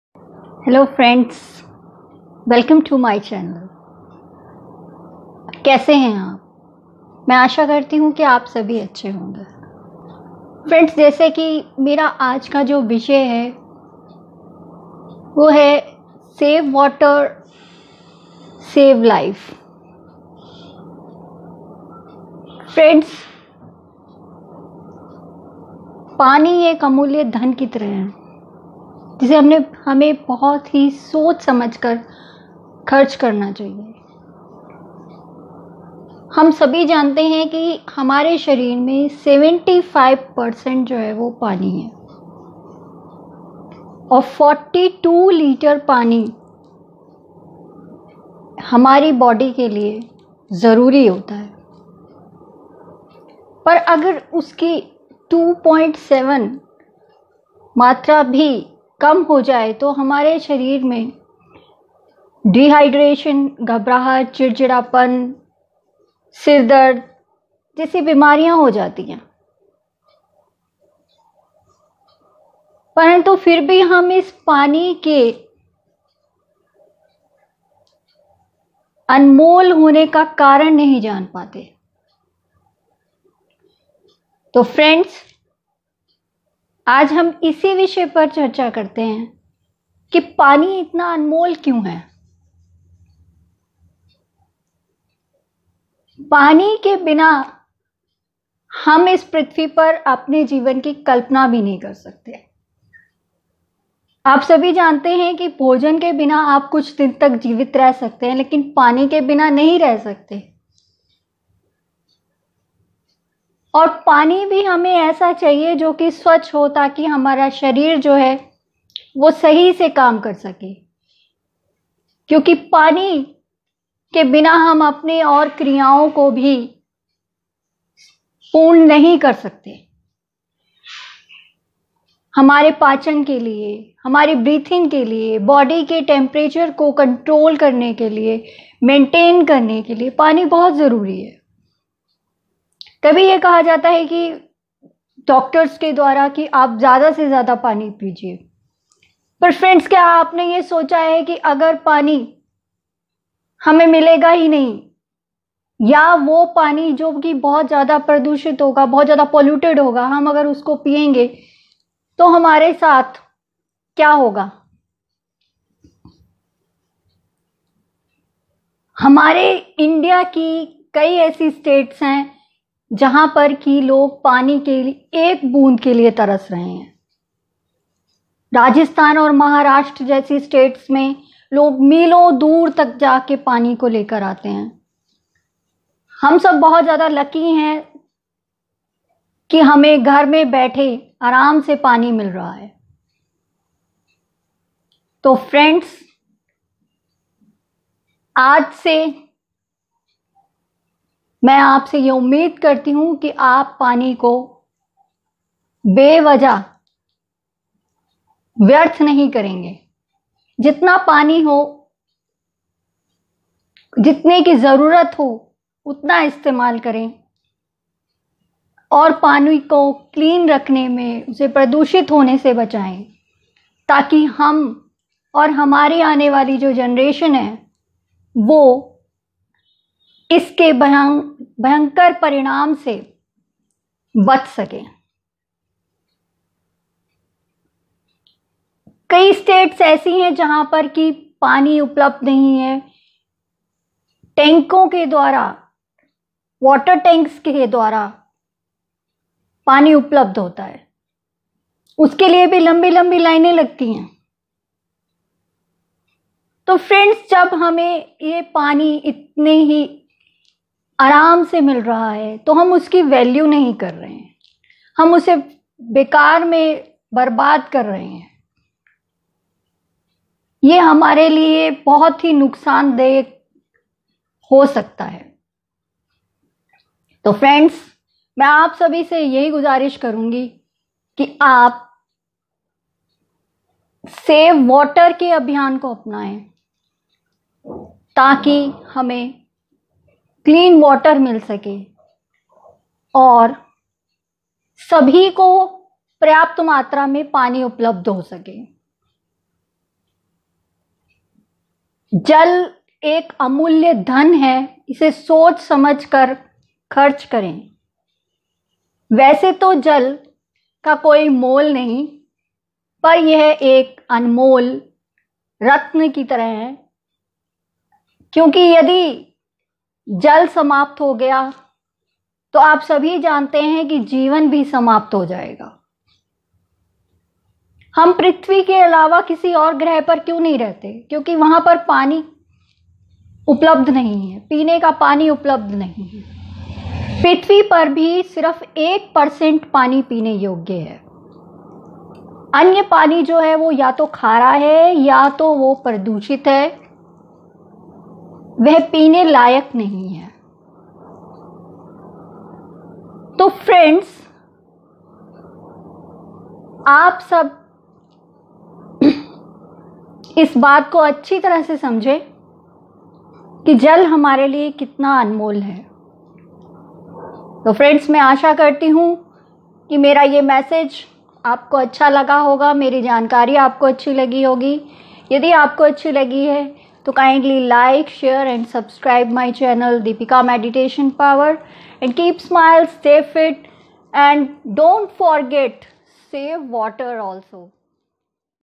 SAVE WATER, SAVE LIFE BY DEEPIKA
Save water , save life = In this video friends we will discuss why water is very important for us.

Meditation is Universal power and best Medication for all. Do Meditation and keep you cool and calm.